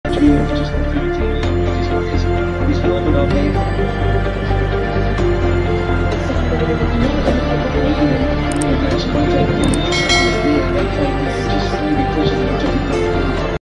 Rovman Powell has Done Extremely Well Not to Completely Wipe Out These Two Kids
😳Rovman Powell, giving chase at full speed, has done extremely well not to completely wipe out these two kids 😳